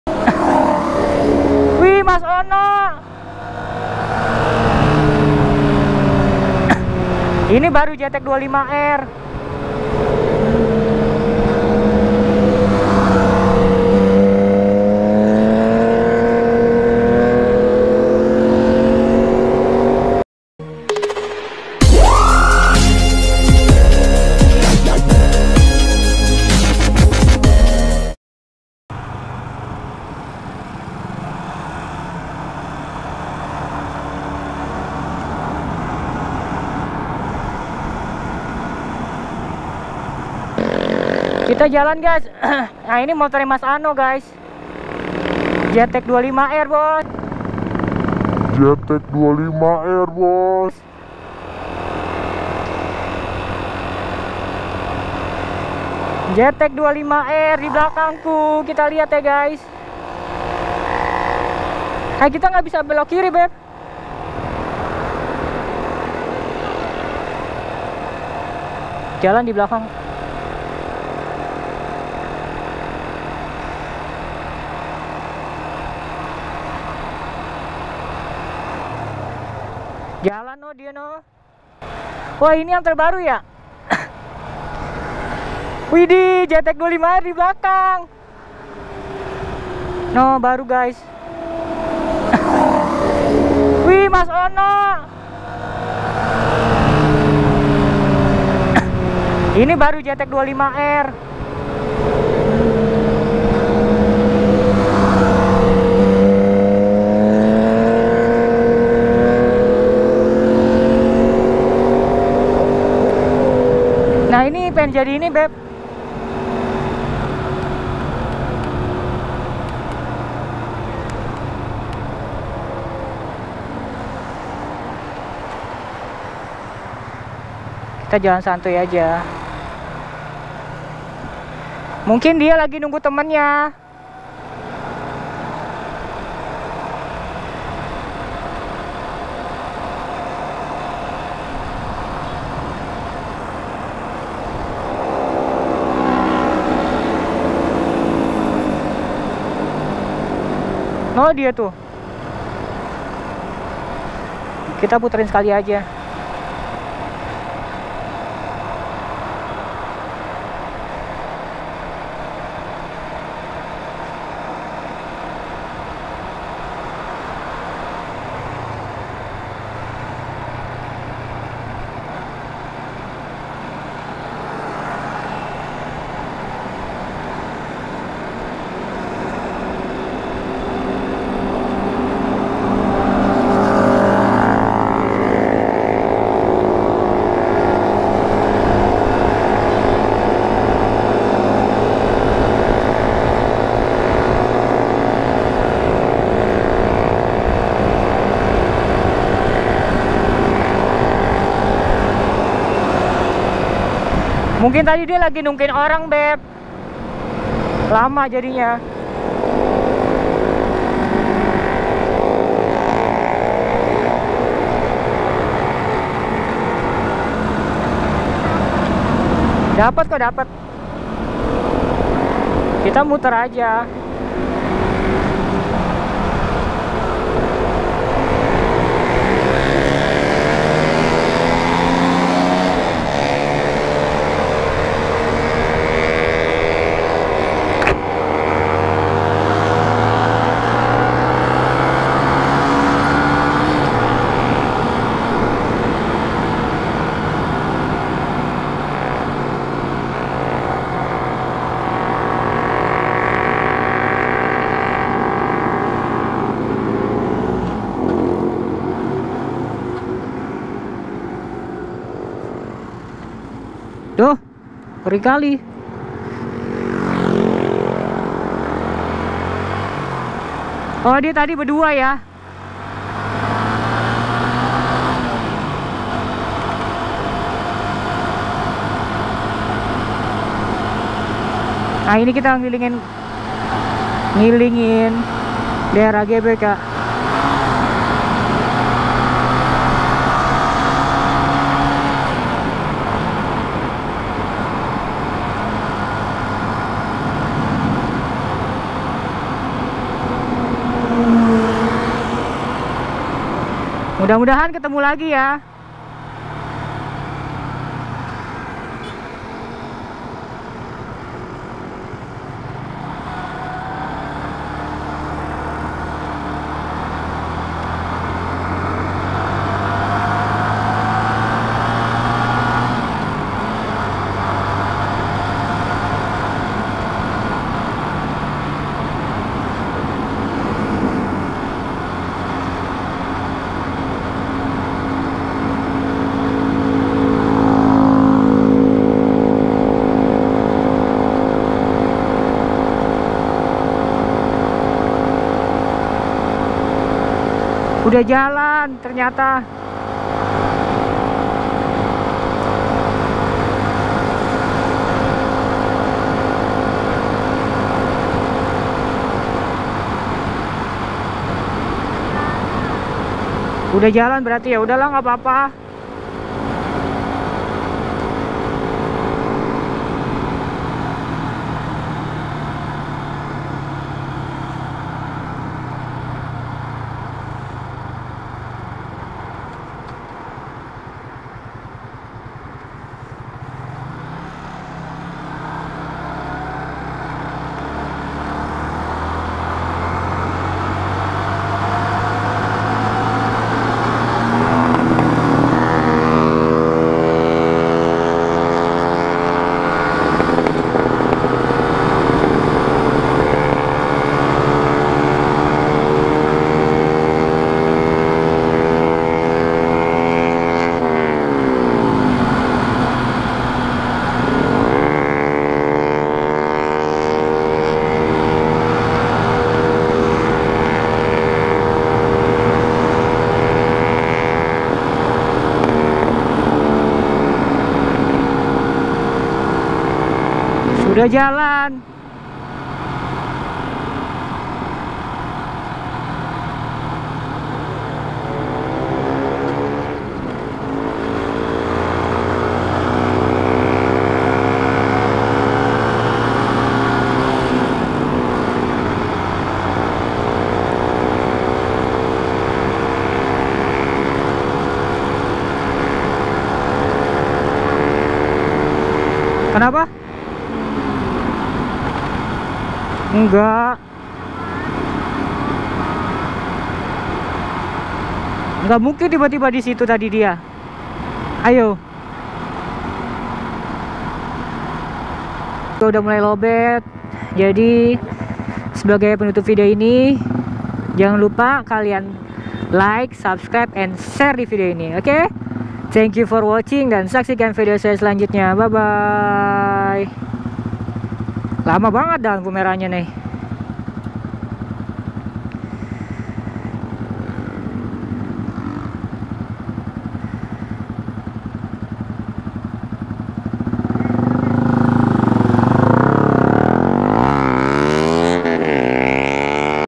DUO ZX25R MAS ANO DAN MUHALDILANA SUNMORI DI SENAYAN
Asli suara ZX25R dari motor orginal / standart ketutupan banget dengan yg racing.
Jadi aman kalau barengan dikira suaranya racing.

Jangan lupa support terus yaa kawan channel ini sampai berkembang.